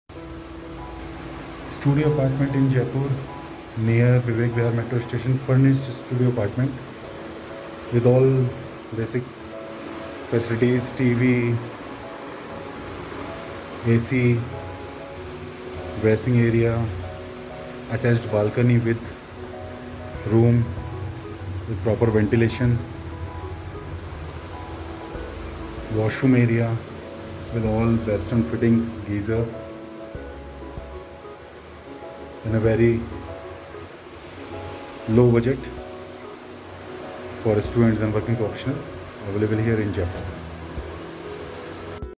Studio Flat for rent in Jaipur | Ramnagar | For Bachelors | 1-month Security Deposit
Here is a video tour of the Nestaway House - 233449 located at Ramnagar, Jaipur. This house is ideal for bachelors looking for a studio apartment for rent in Ramnagar, Jaipur.

Other Amenities: Furnished, Air Conditioner, Mattress etc